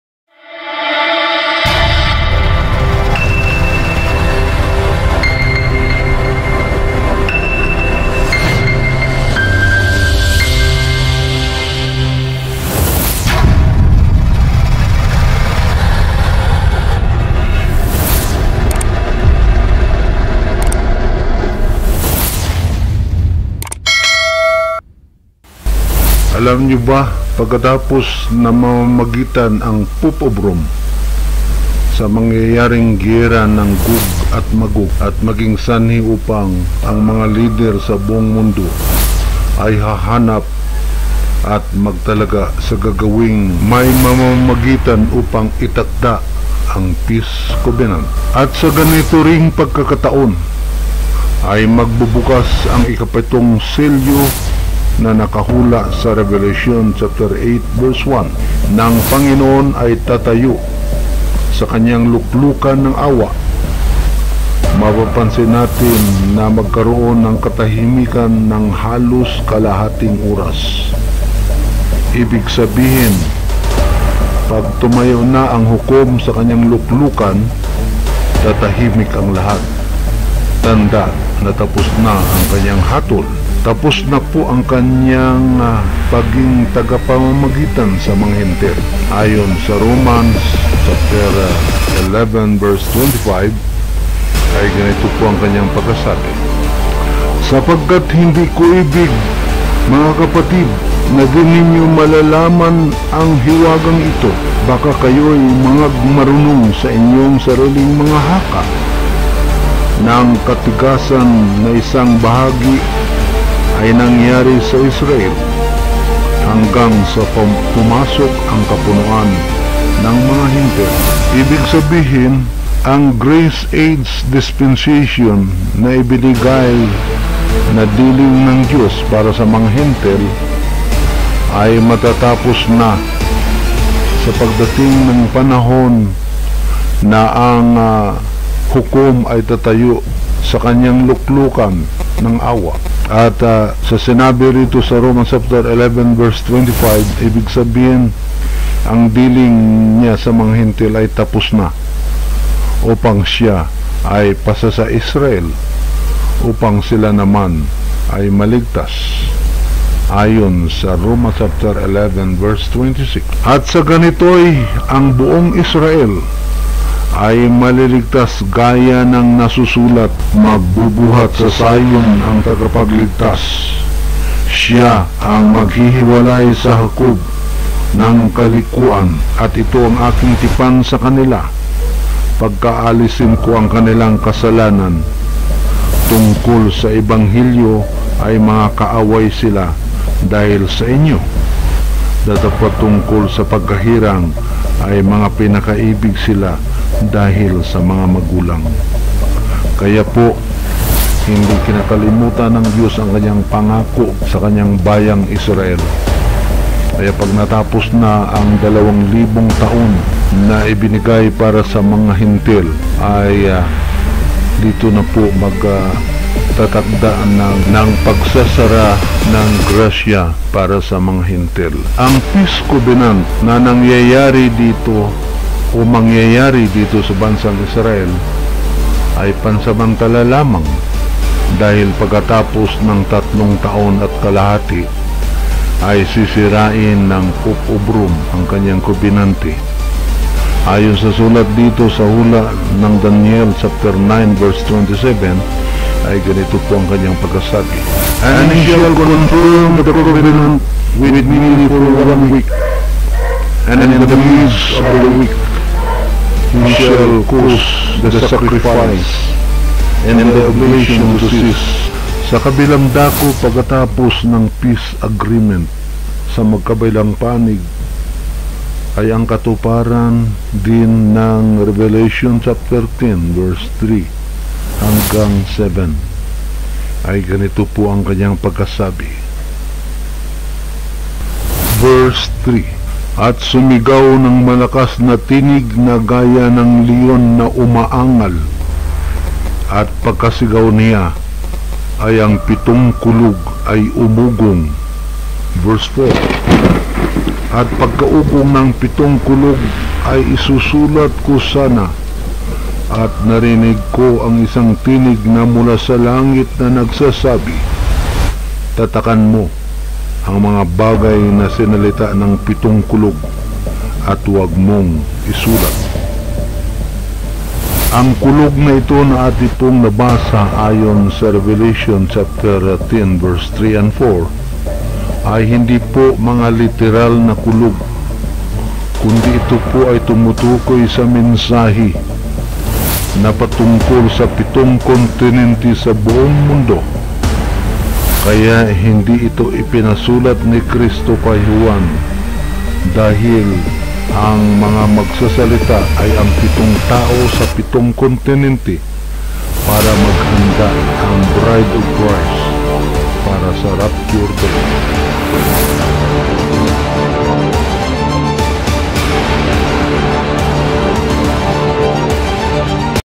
THE OPENING OF THE 7 THUNDERS
THE THUNDERS IS THE MESSAGE TO THE 7 CONTENENT BEFORE THE RAPTURE.

Revelation 10:4
And when the seven thunders had uttered their voices, I was about to write: and I heard a voice from heaven saying unto me, Seal up those things which the seven thunders uttered, and write them not.

Revelation 10:3
“And cried with a loud voice, as when a lion roareth: and when he had cried, seven thunders uttered their voices.